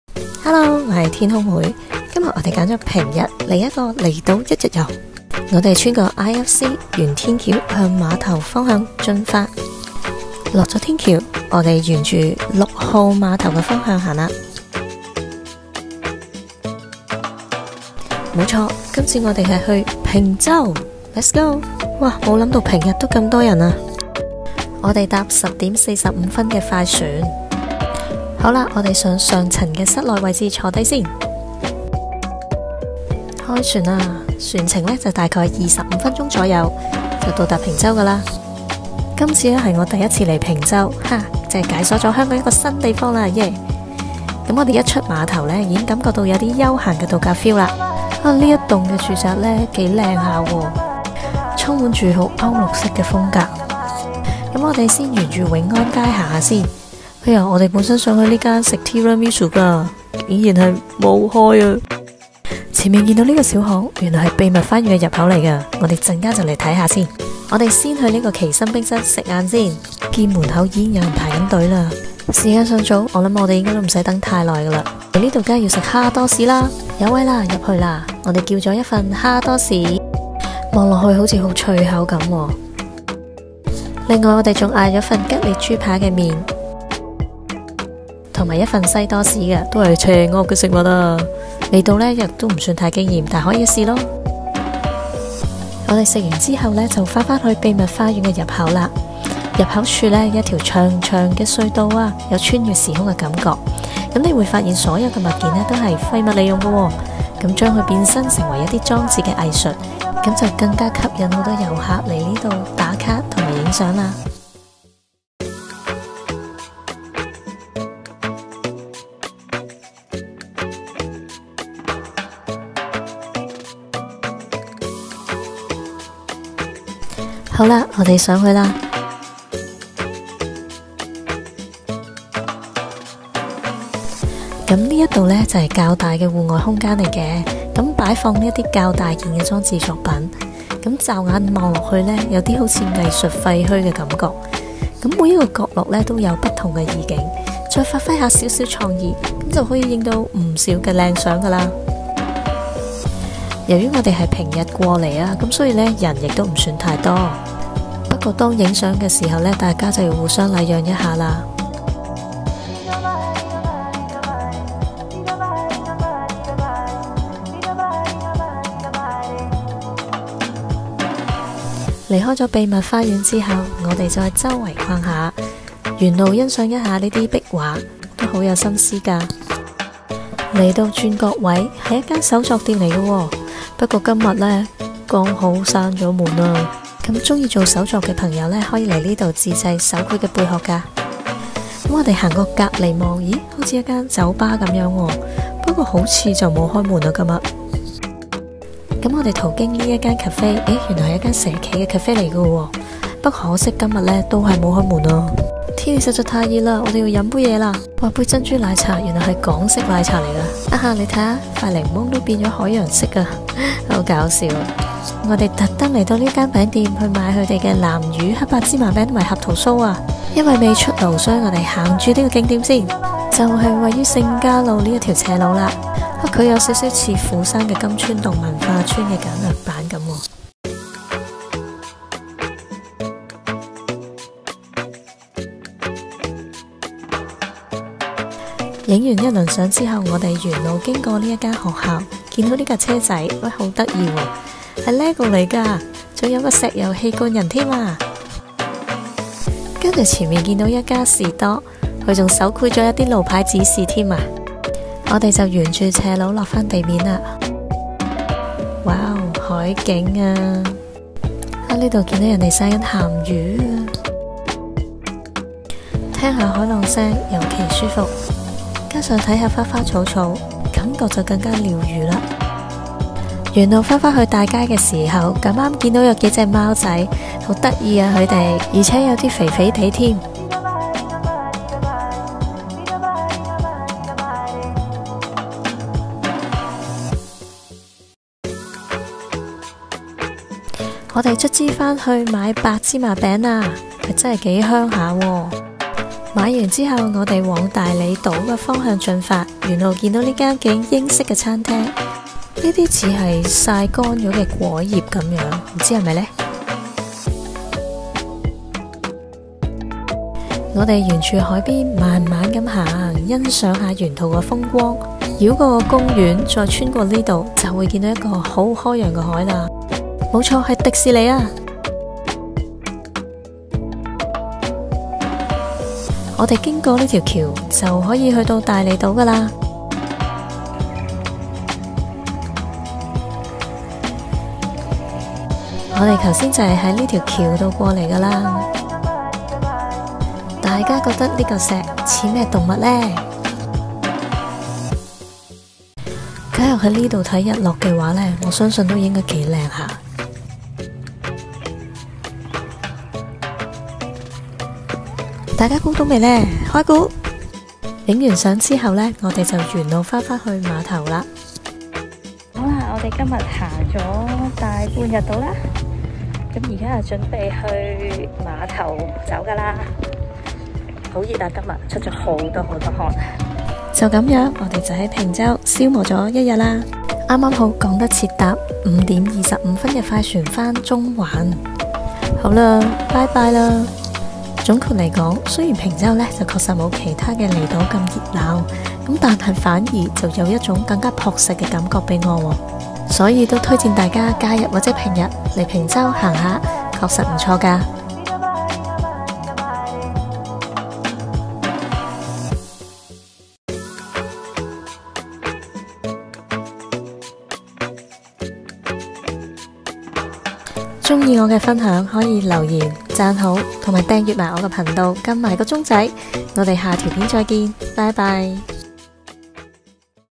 【本港周圍走】│坪洲│永安街│祺森│蝦多士│秘密花園│南乳芝蔴餅│聖家路│大利島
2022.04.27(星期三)

今天和朋友來個坪洲遊
我從未踏足過坪洲, 所以藉著今次
可以感受一下旅行的感覺了~

我們在中環6號碼頭坐船

這裏雖不及其他離島大和熱鬧
但卻有份樸實感給我
不過平日來, 有樣東西不能預計的是...
很多店子可能會剛好休息~

所以朋友們若想來, 就要取捨
想來看熱鬧的坪洲
還是想避開人潮, 感覺一下離島的恬靜
甚或乎來2次, 咪得囉~!